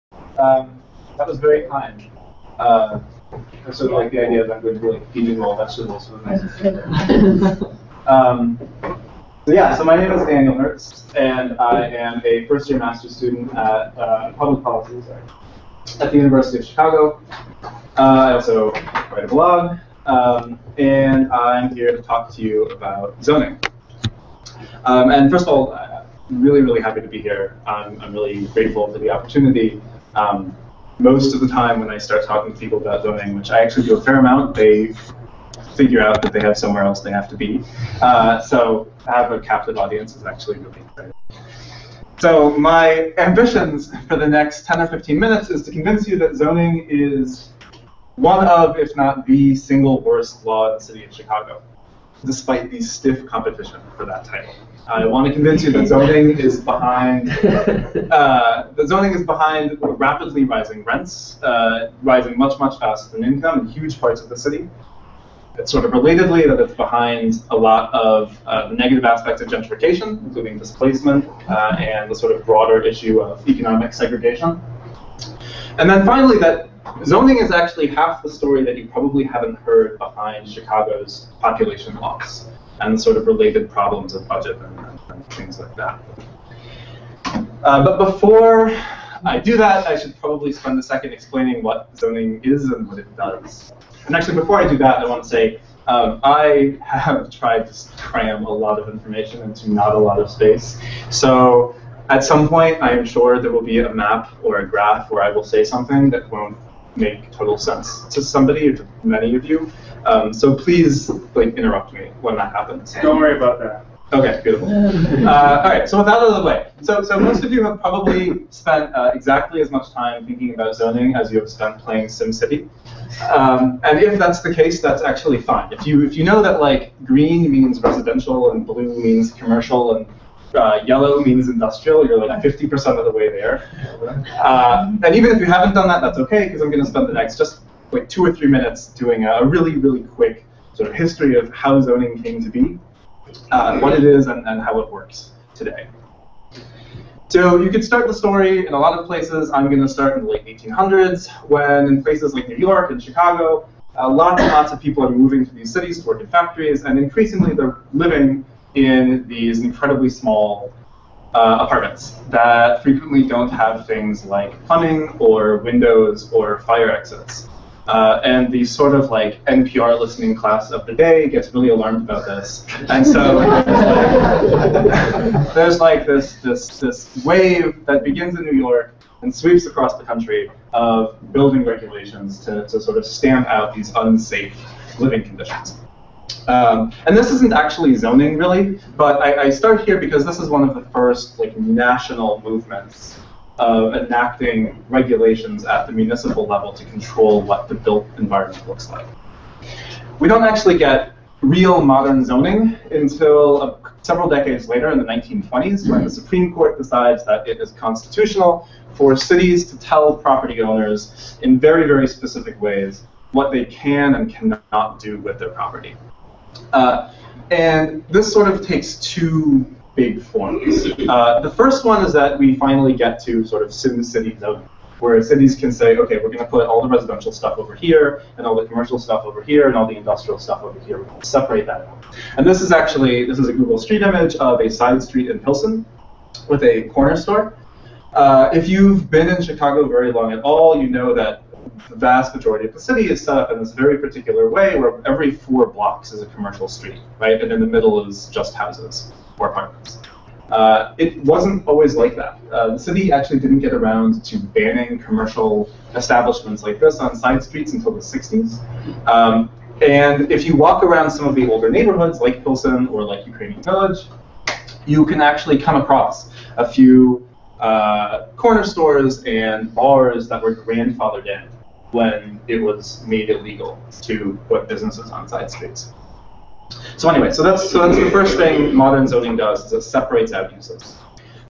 All About Zoning: Part 1 - Origins of Zoning
Urban policy blogger Daniel Hertz will talk about the downsides of low-density zoning in Chicago, and how he uses open zoning data to understand the built environment.